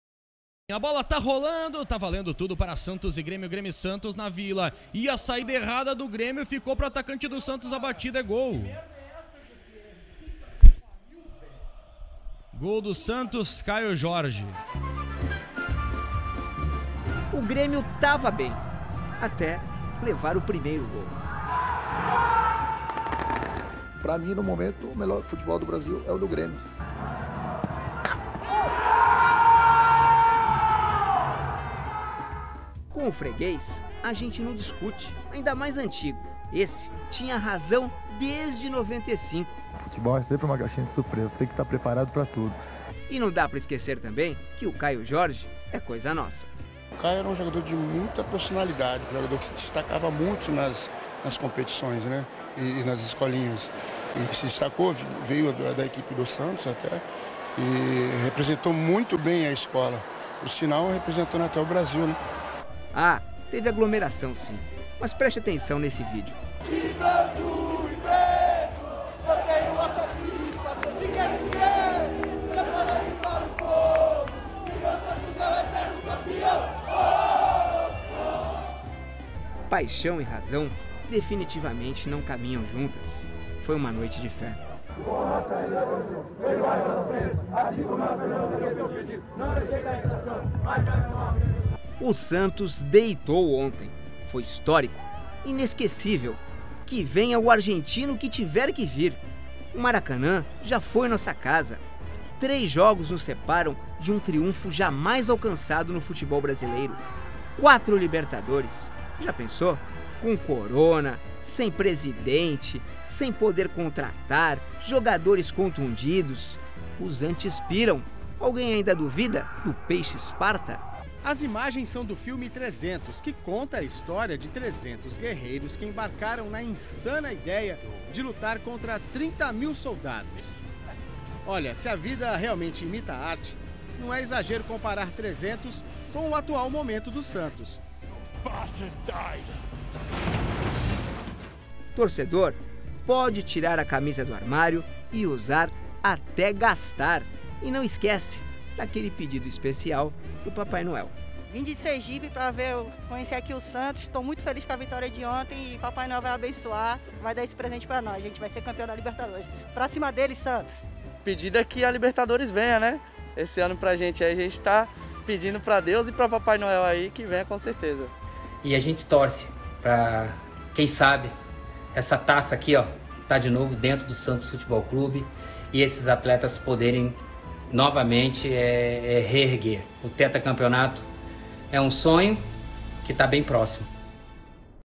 O Santos atropelou o Grêmio. Decidimos mostrar como as redes sociais enxergaram a histórica goleada.
O Santos atropelou o Grêmio. Passou o trator, amassou, esmagou. E agora? O que dizer sobre a "Gigantesca vitória" quase 24 depois? Decidimos mostrar a partida por ângulos diferentes.